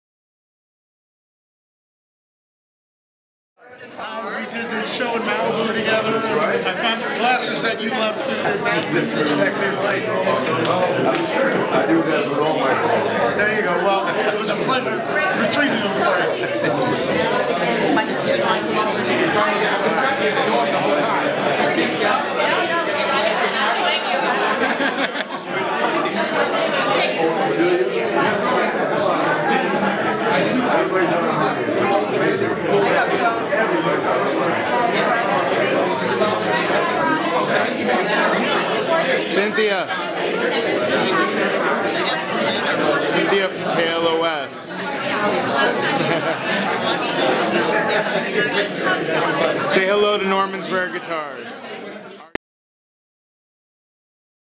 Dick Van Dyke and Cynthia Fox at the MidNight Mission Golden Heart Award
Dick Van Dyke an American Icon and Cynthia Fox from KLOS at the Midnight Mission Golden Heart Award on May 10, 2010. They were with Norman Harris from Norman's Rare Guitars.